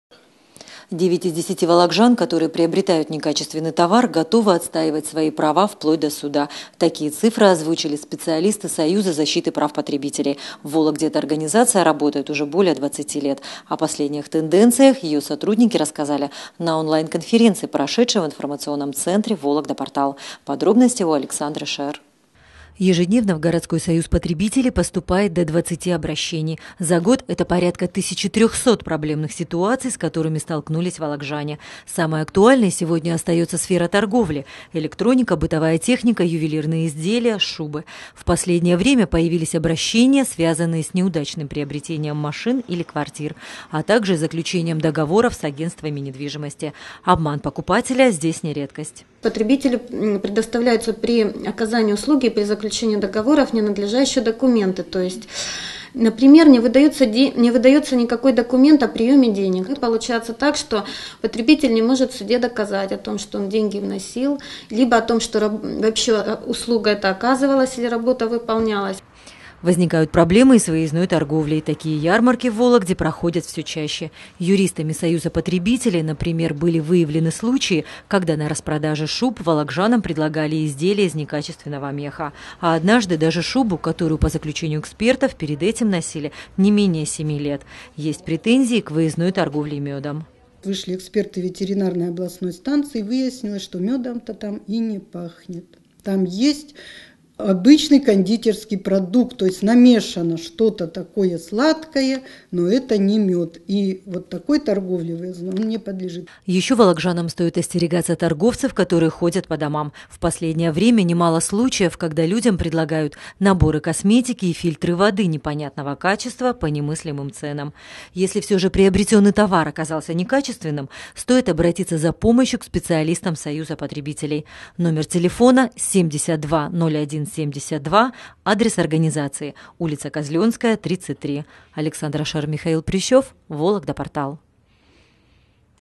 9 из 10 вологжан, которые приобретают некачественный товар, готовы отстаивать свои права
Такие цифры обнародовали специалисты Союза защиты прав потребителей на онлайн-конференции, которая прошла в информационно-издательском центре «Вологда-Портал». В Вологде эта организация работает уже более 20 лет. Ежедневно сюда поступает до 20 обращений. За год -- порядка 1300 проблемных ситуаций, с которыми сталкиваются вологжане.